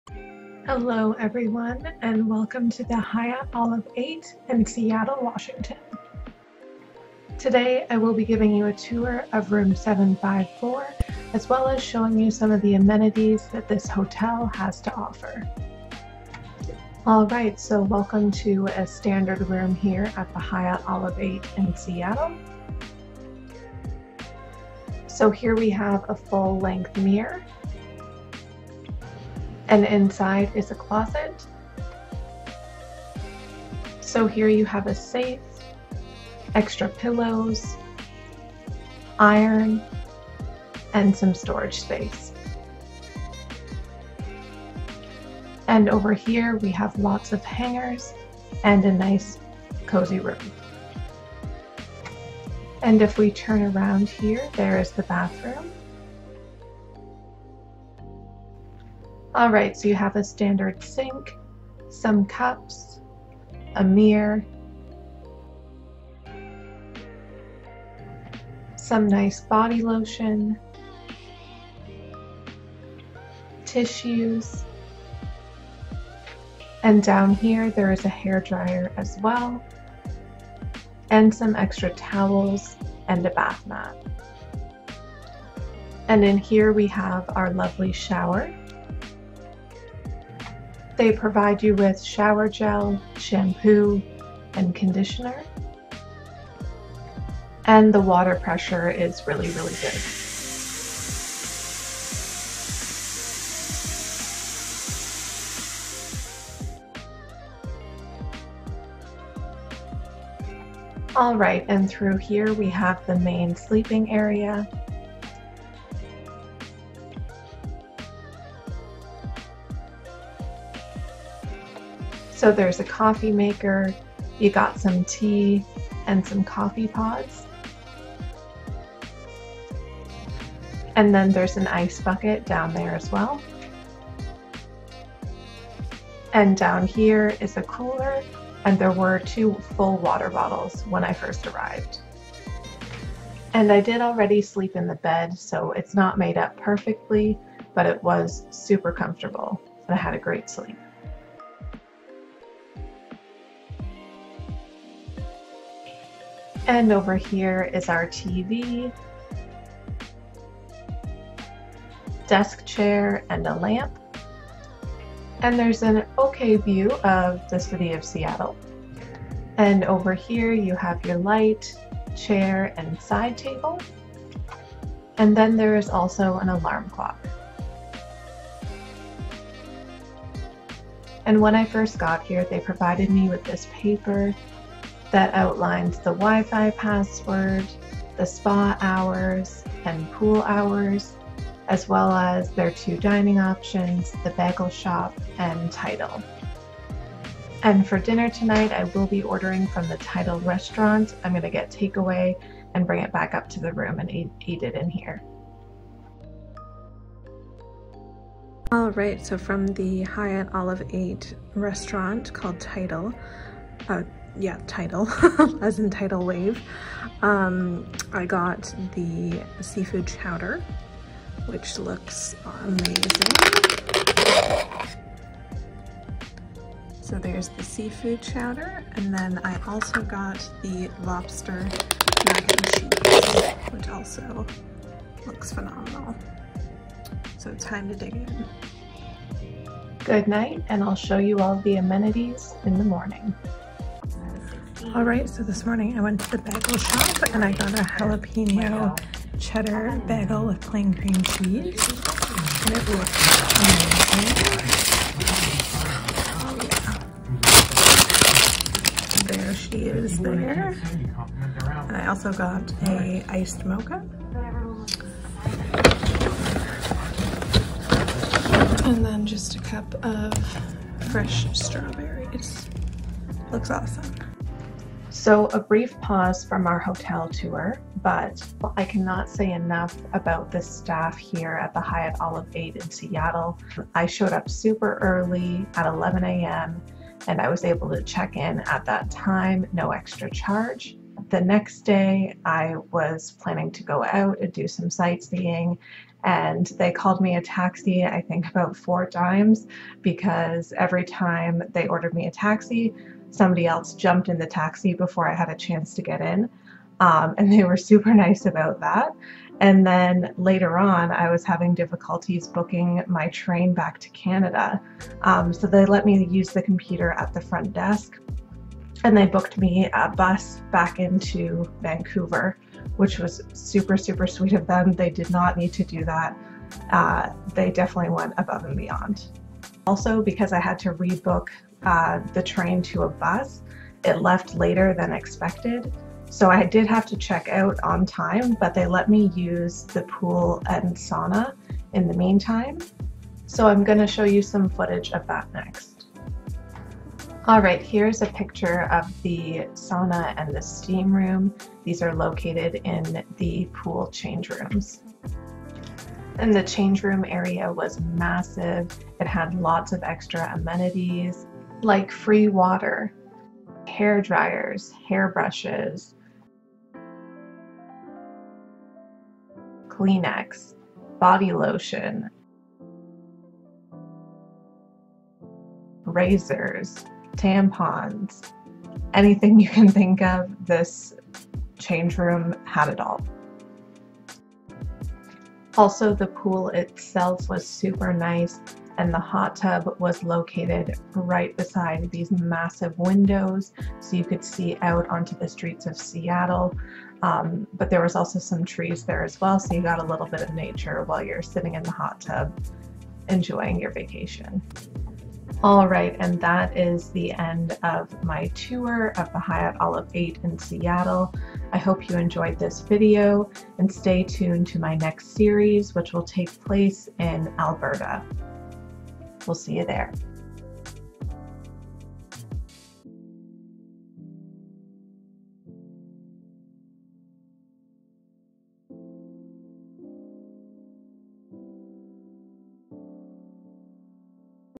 Seattle Hyatt Olive 8 Hotel Review | Pre Cruise Hotel | Post Cruise Hotel | Solo Alaska Cruise
Today I checked into the Hyatt Olive 8 hotel in Seattle. This was my post cruise hotel of choice for an 8 night Alaskan adventure with Royal Caribbean onboard Ovation of the Seas.

The staff at this hotel were absolutely wonderful and went above and beyond to ensure my stay was pleasant and enjoyable. I can't thank them enough for their hospitality.